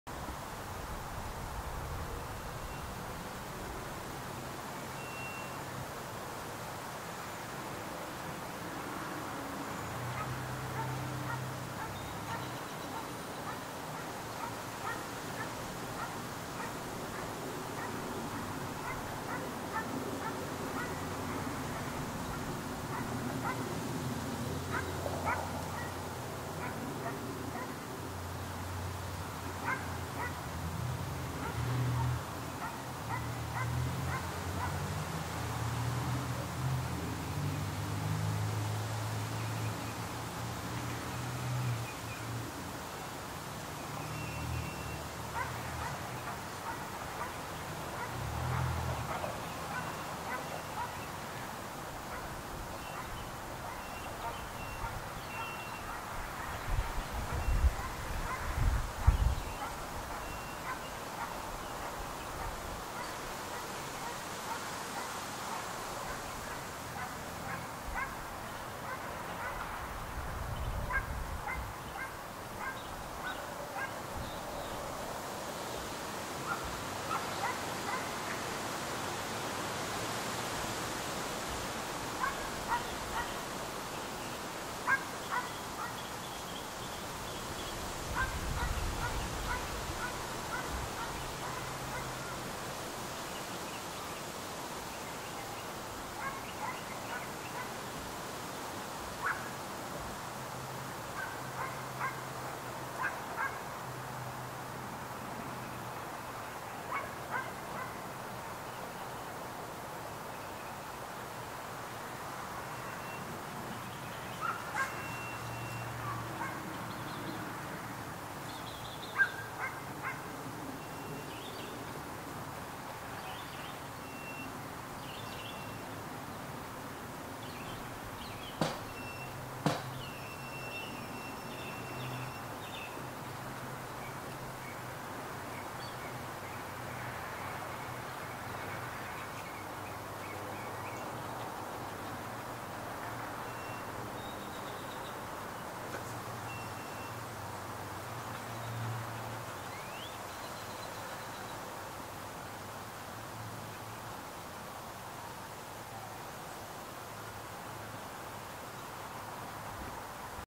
Shadow Play (4-12)
Branches casting shadows on the fence in the evening. There just seems to be something captivating about the quality of the light at dusk. Of course, sometimes I just seem to be captivated by the most mundane things. :/